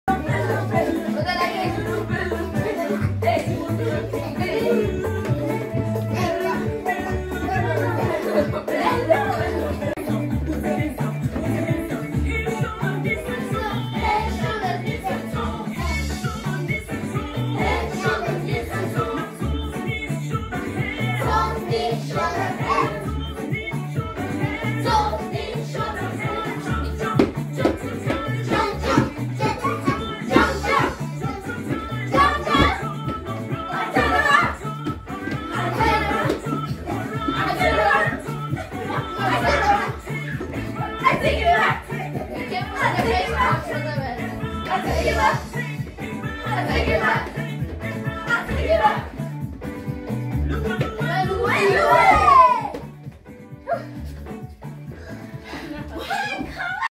Dancing and celebration. Can you dance like us??????
Celebrating our father, our God, our creator. Join us and dance with us.
Put on your dancing shoes
Own right to the background song